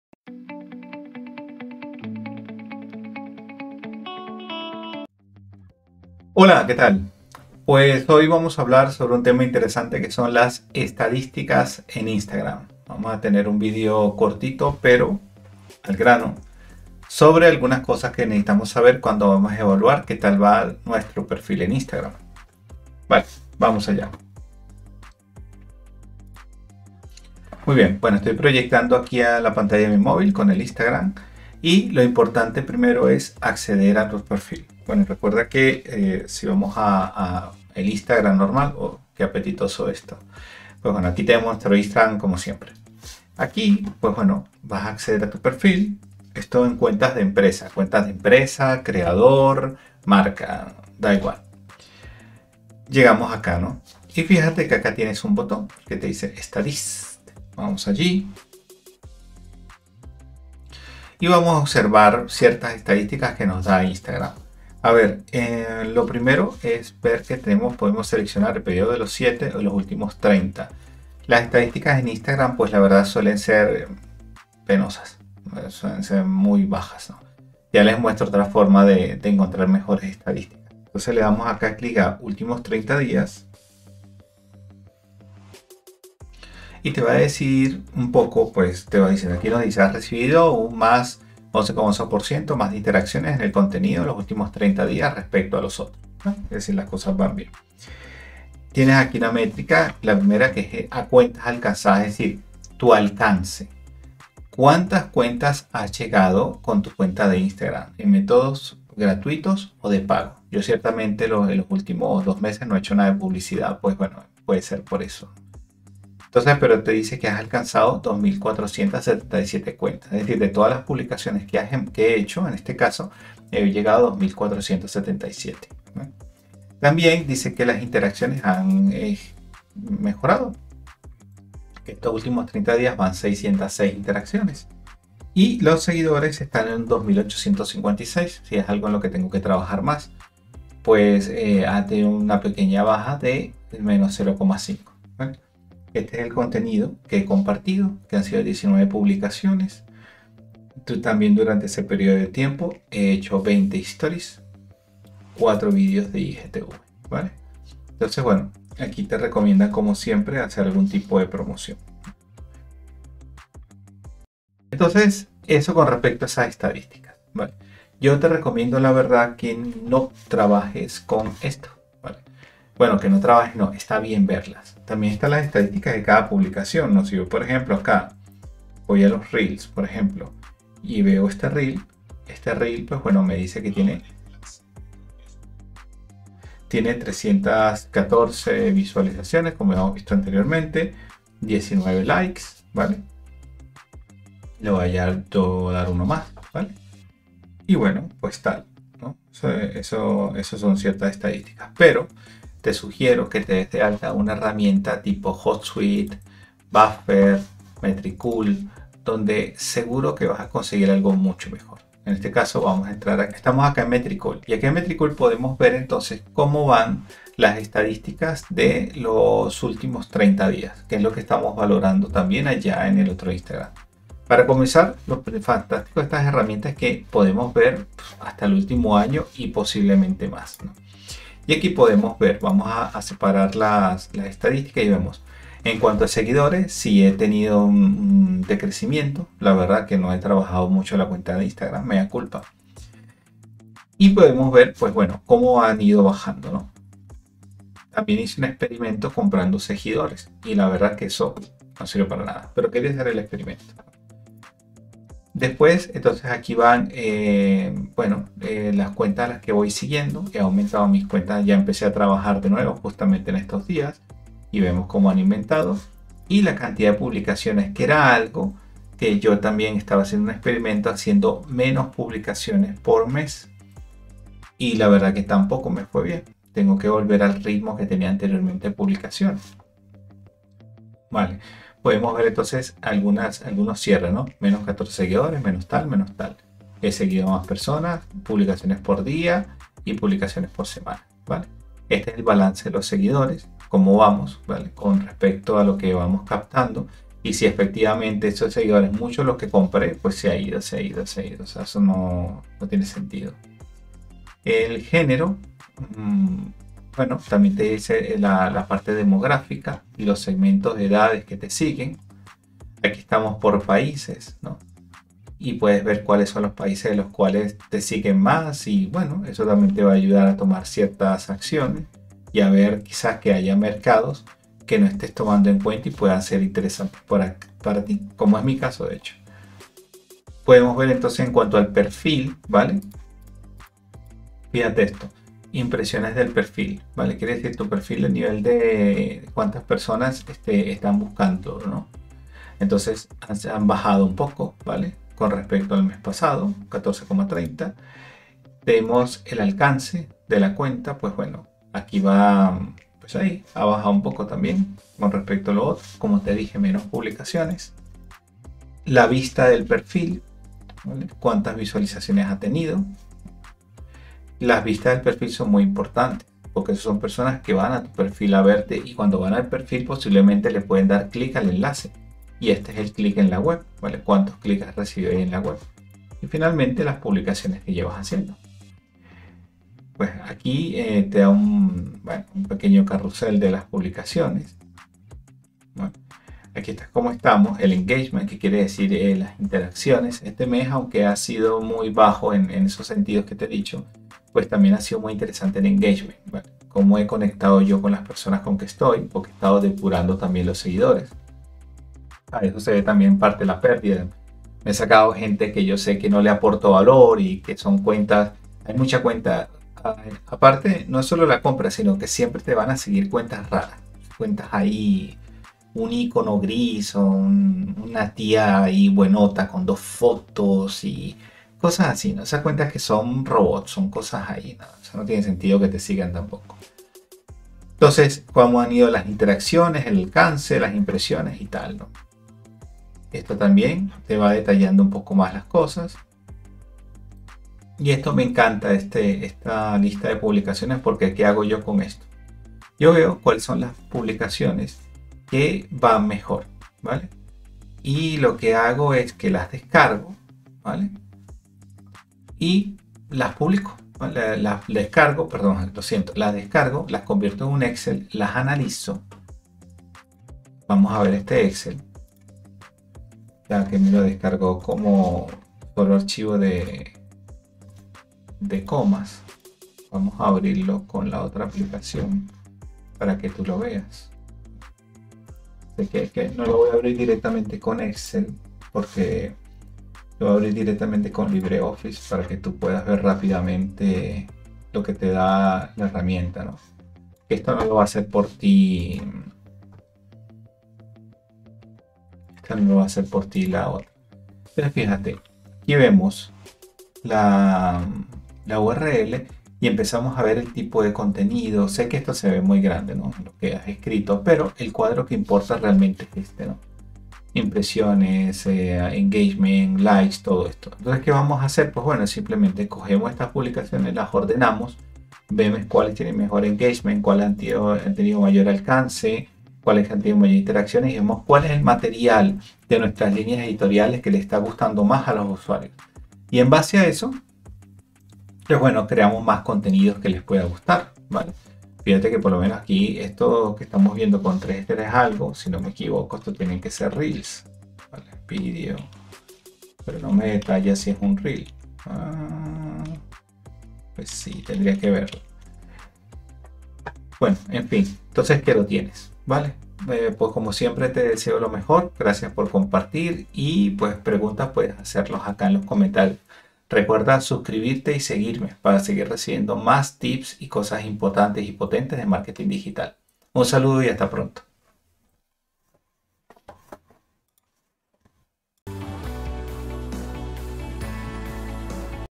Estadísticas en Instagram 📈 | LO QUE DEBES SABER
Las estadísticas de instagram son bastante escasas. En este vídeo valoramos cómo sacarles el mayor provecho.

CAPÍTULOS
0:00 Introducción del vídeo
0:27 Accede al perfil de Instagram
0:52 Accede a las estadísticas en Instagram
1:04 Período de tiempo de las estadísticas
1:25 Información general de las estadísticas
1:38 Cuentas alcanzadas
2:05 Interacciones con el contenido
2:15 Total de seguidores
2:25 Contenido que se ha compartido
2:44 Otras herramientas recomendables
3:32 Estadísticas de Instagram usando Metricool
3:51 Estadística de seguidores en Metricool
4:14 Estadística de seguidos en Metricool
4:26 Estadística de publicaciones en Metricool
4:41 Cierres de estadísticas
4:51 Balance de seguidores en Metricool
5:05 Estadísticas de seguidores por género, edad, ciudad y país
5:30 Impresiones del perfil de Instagram
5:48 Alcance de la cuenta de Instagram
5:58 Vista del perfil
6:12 Clics en la web y publicaciones
6:26 ¿Qué es el Engagement en Metricool?
7:08 Otros elementos: interacciones, alcance, impresiones, publicaciones
7:18 Lista de publicaciones en Metricool
8:14 Visualizar lista de publicaciones de Instagram en Excel
9:08 Conclusión del vídeo

😊 Si tienes dudas puedes hacer tus preguntas en los comentarios de este vídeo.

📝 Guión, grabación y edición de Otto Duarte: Coach Digital - Docente y Growth hacker.